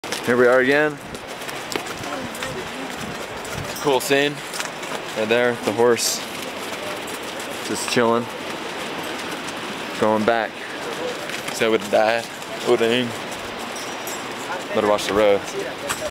Inca Trail Horse Ride Part II
Same ride same purpose in going back to base camp. Because of my elevation sickness. Dang it was a long horse ride. Not that you want to know but I received a huge bruise from riding the Peruvian horse.